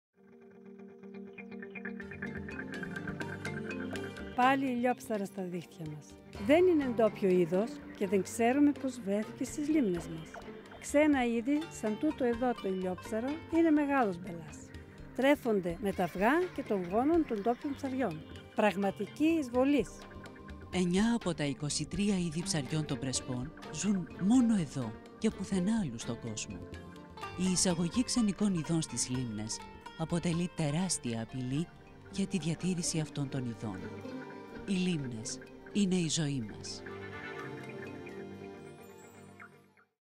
"Πραγματικοί εισβολείς" - ΕΤΑΙΡΙΑ ΠΡΟΣΤΑΣΙΑΣ ΠΡΕΣΠΩΝ
Ως "πραγματικούς εισβολείς" χαρακτηρίζει η Τούλα Χριστιανοπούλου, μία από τις λίγες γυναίκες που ψαρεύουν επαγγελματικά στις Πρέσπες, τα ξενικά είδη που έχουν εισβάλει στις λίμνες των Πρεσπών. Η πρωταγωνίστρια του τηλεοπτικού σποτ επισημαίνει τις επιπτώσεις τους στα ντόπια και μοναδικά στον κόσμο ψάρια της Πρέσπας.

Τα ξενικά είδη αποτελούν μία από τις μεγαλύτερες απειλές για τη διατήρηση των ενδημικών ψαριών της Πρέσπας, καθώς τρέφονται με τα αυγά και το γόνο των ντόπιων ψαριών.

Το τηλεοπτικό σποτ προβάλλεται στην ελληνική τηλεόραση στα πλαίσια του προγράμματος LIFE+ Ενημέρωση & Επικοινωνία (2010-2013): "Ψάρια, Αλιεία και Ευρωπαϊκή Πολιτική στη λεκάνη των Πρεσπών". Το πρόγραμμα συγχρηματοδοτείται κατά 50% από την Ε.Ε.